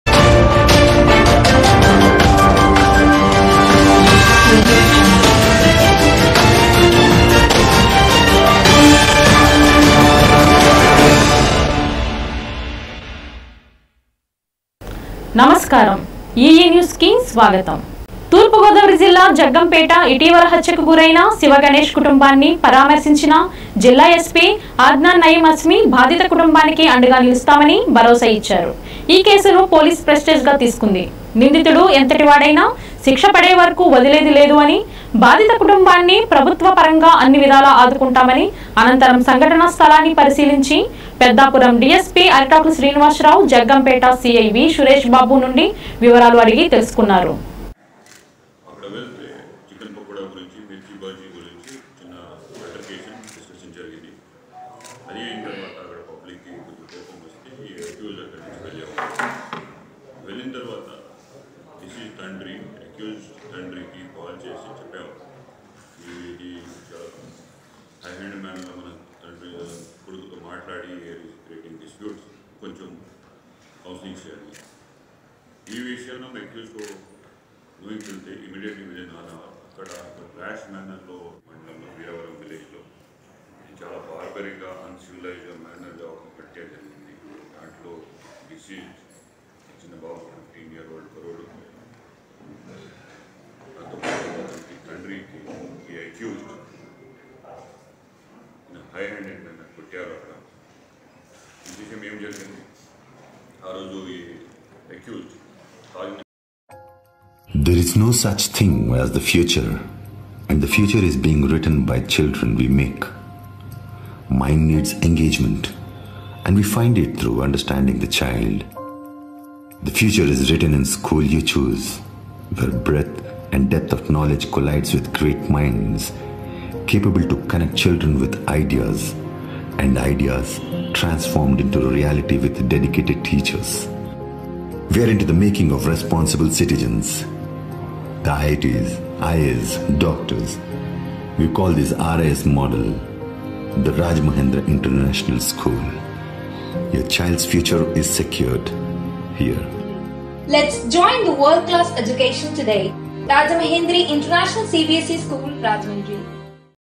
తూ.గో. జిల్లా జగ్గంపేట ఇటీవల హత్యకు గురైన శివ గణేష్ కుటుంబాన్ని పరామర్శించిన sp అద్నాన్ నయిం అస్మి
తూర్పుగోదావరి జిల్లా జగ్గంపేట ఇటీవల హత్యకు గురైన శివ గణేష్ కుటుంబాన్ని పరామర్శించిన జిల్లా ఎస్పీ అద్నాన్ నయిం అస్మి